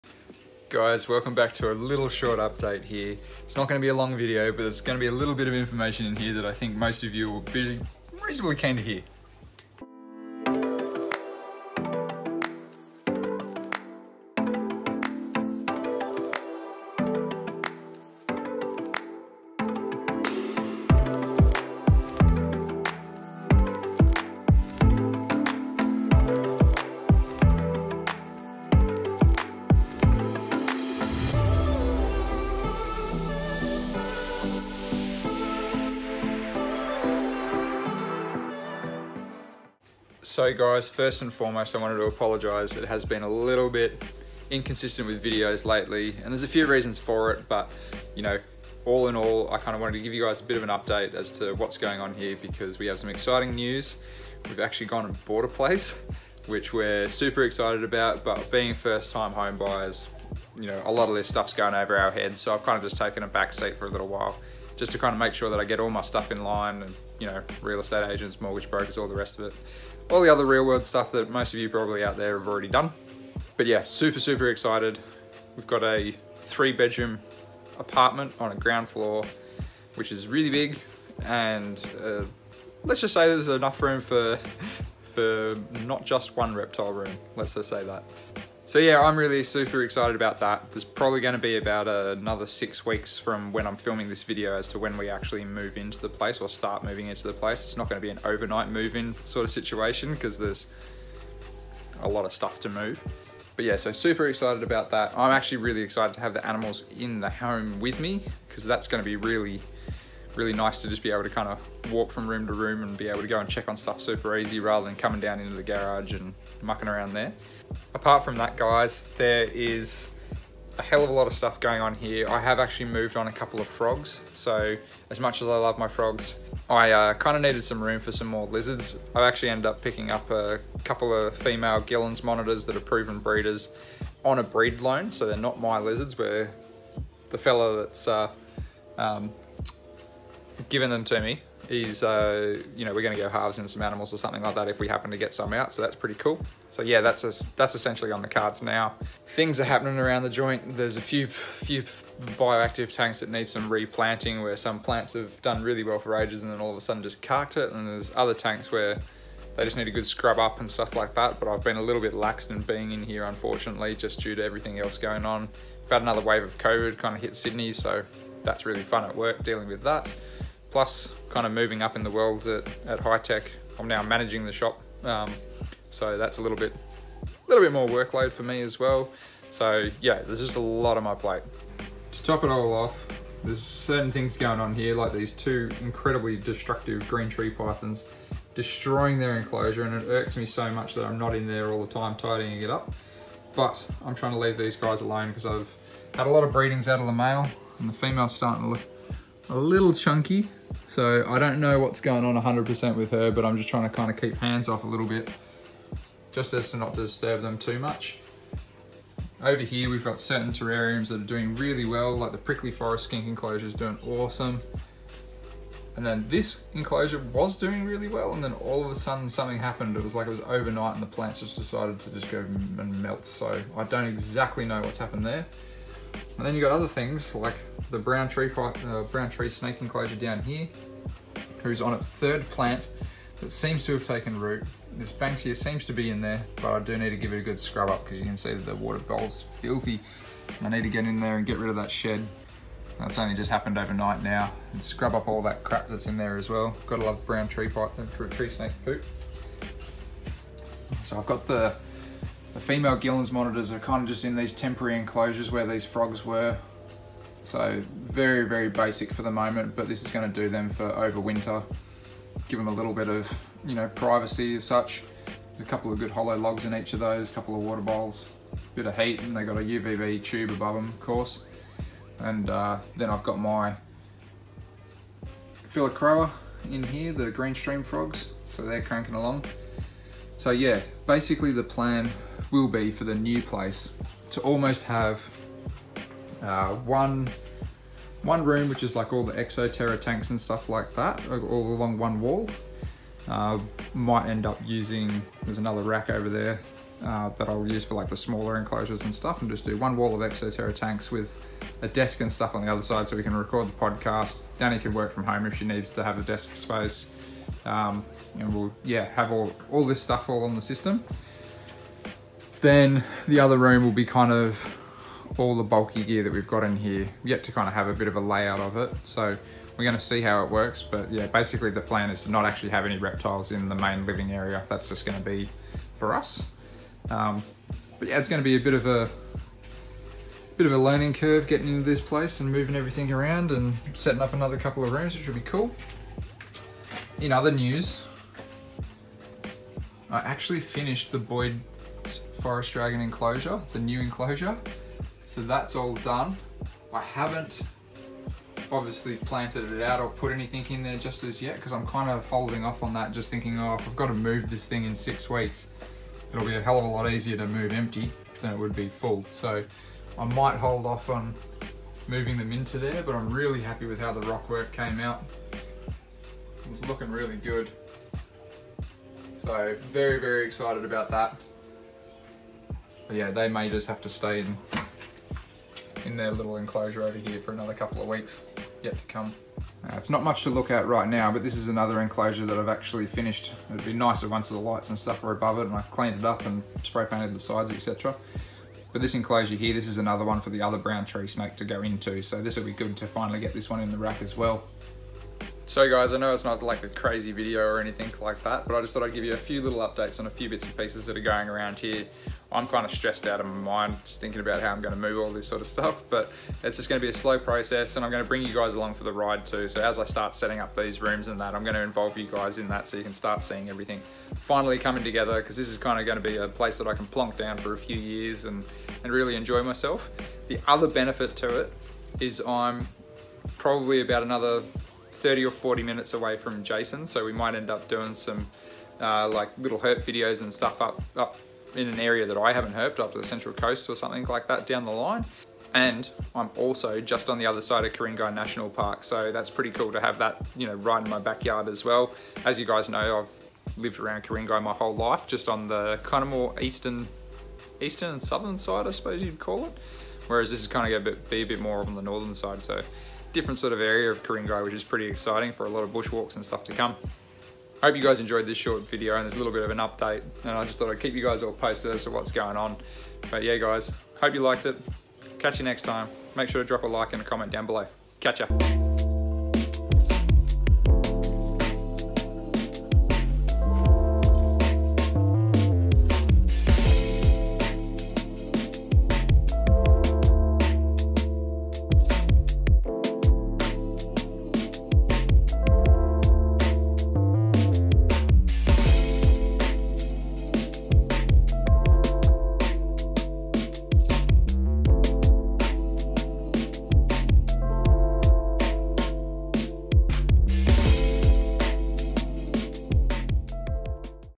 We’re Moving! Plus updates on the gilleni army and the Boyd’s forest dragon build!
Make sure to check me out on my other social network accounts below:

Come and listen to Jason & I on The Australian Herpetoculture Podcast on iTunes and Spotify!

Check out the podcasts from the Morelia Python Radio Network @ moreliapythonradio.com